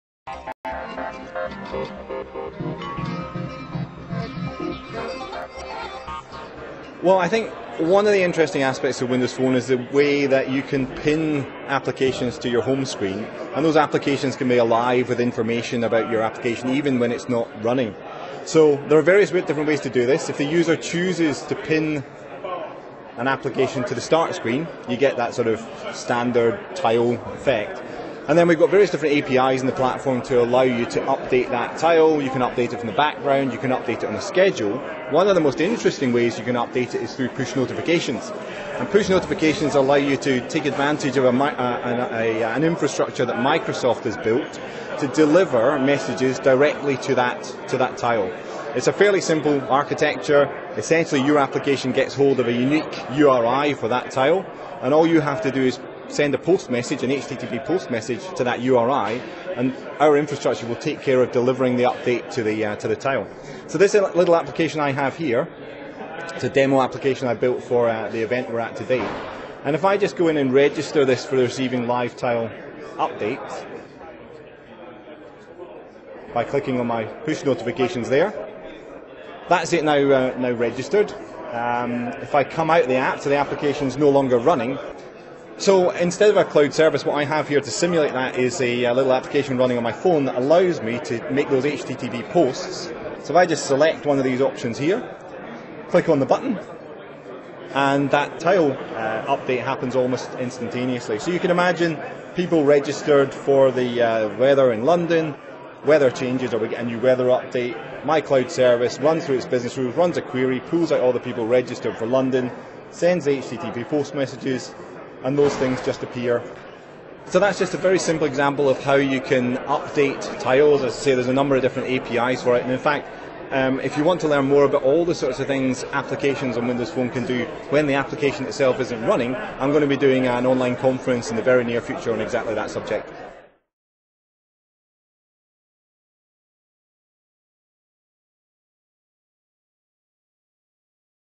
2 Minute demo on Live Tiles on Windows Phone
Find out how to use push notifications to update live tiles on Windows Phone with Mike Ormond at devweek